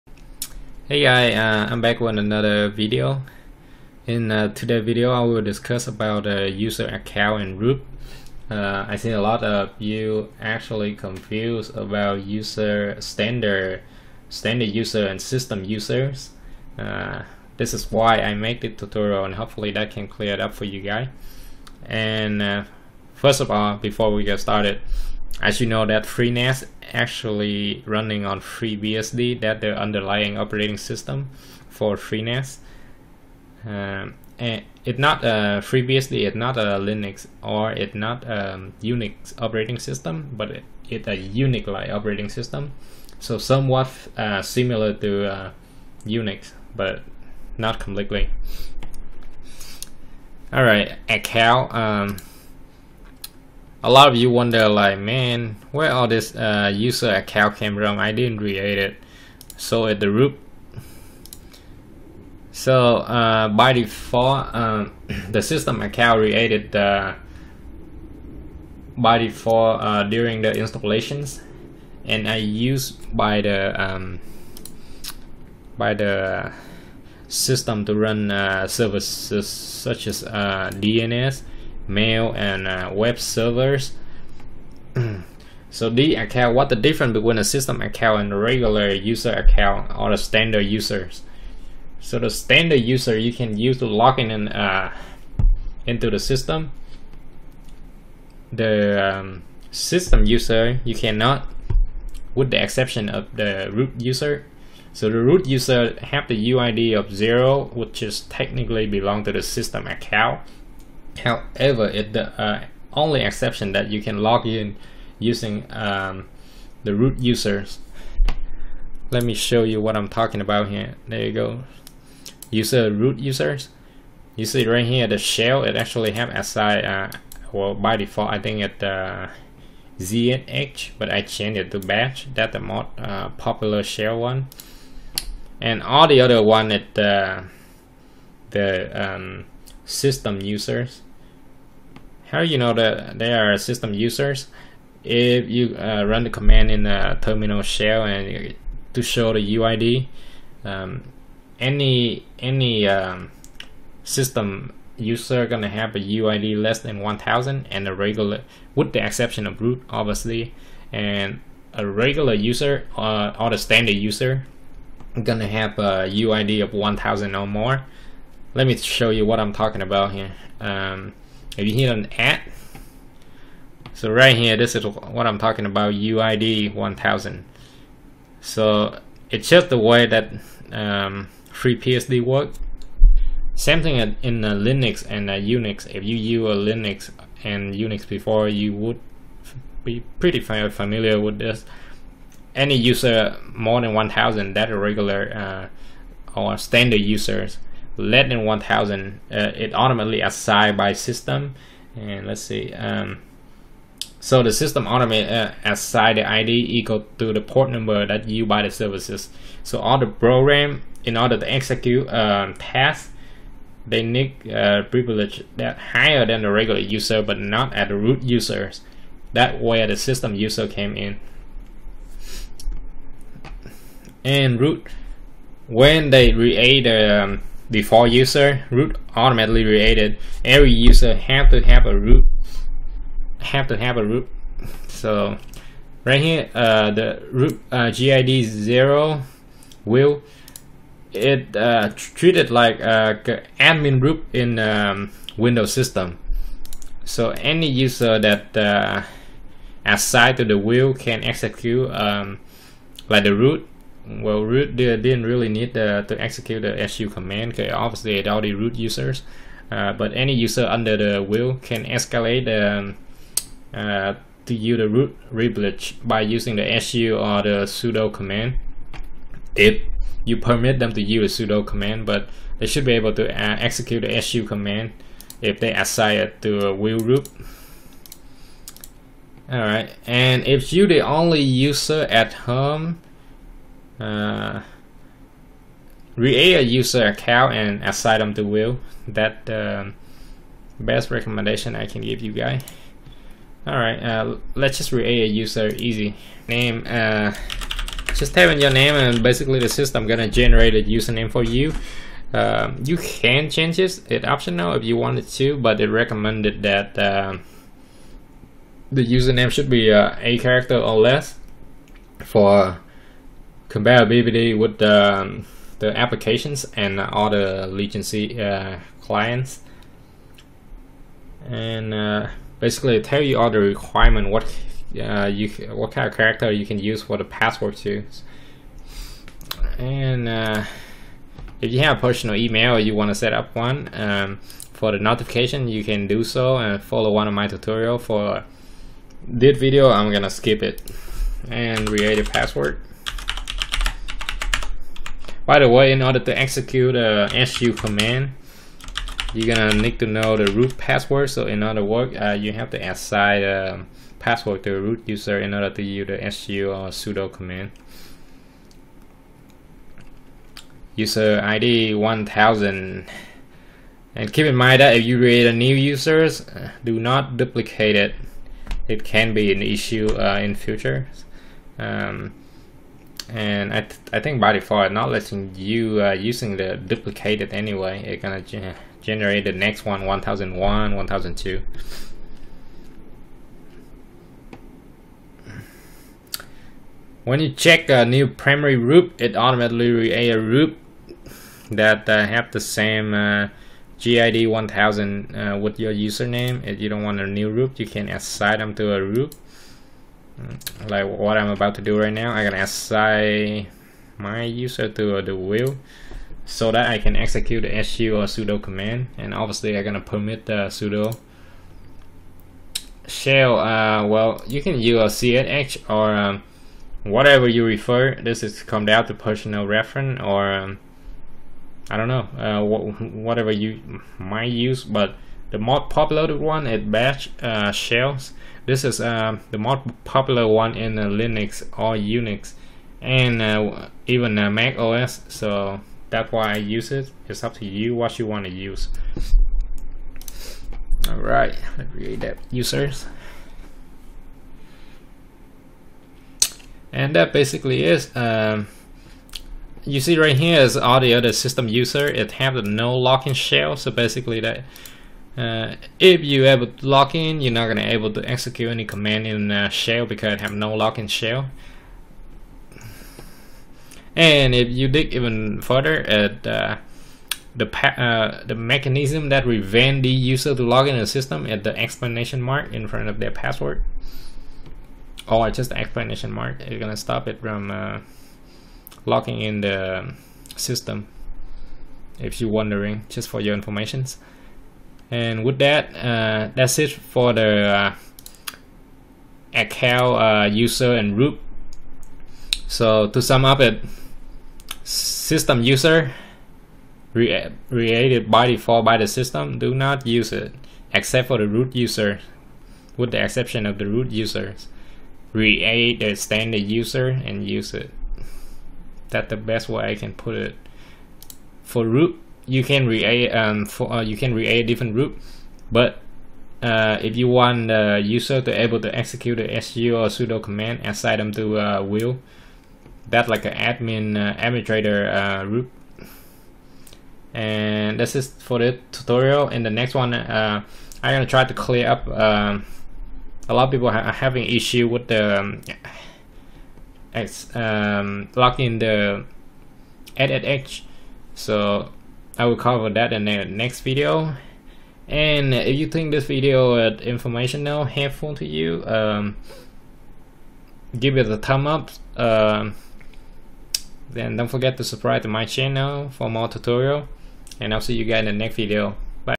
FreeNAS 11.2 - Users and Basic Account Management
This video will show you the differences between System Accounts and User Accounts (or Standard Accounts) and hopefully it will help you some what understand how FreeBSD (FreeNAS's underlying OS) user accounts work.

🚀 Things I used for my server

🚀 Tools I used

🚀 Devices I used

🚀 Networking/Cybersecurity/Programming Books

🚀 TrueNAS HBA SAS controller IT Mode from the Art of Server

© SYSADMIN102™ LLC